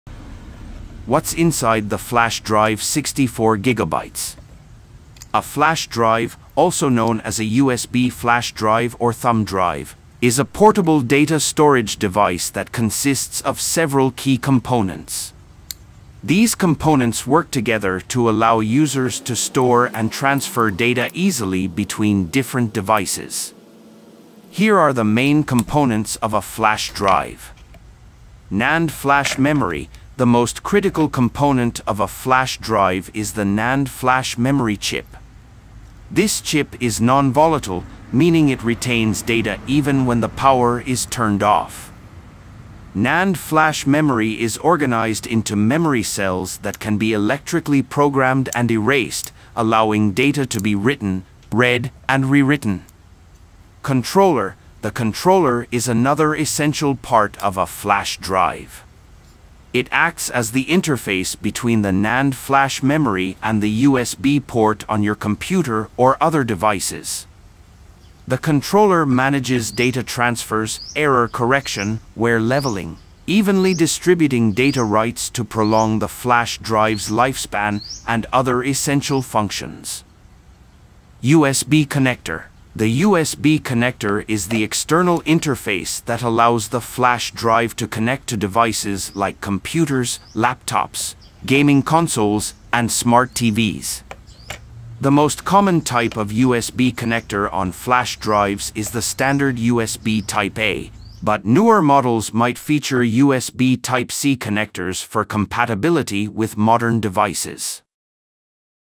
💾🔍 What's Inside the Flash Drive? 64GB SanDisk Teardown Reveals the Secrets!
@MakeGold  Ever wondered what’s hidden inside your 64GB SanDisk flash drive? In this video, we carefully take it apart to uncover the internal components, explore whether there are any precious metals like gold, and explain how flash storage works!

✅ Full teardown of SanDisk flash drive
✅ Inside look at NAND chips and circuit board
✅ Are there valuable materials inside?
✅ Basics of USB flash drive technology

Perfect for tech lovers, recyclers, and the just-curious!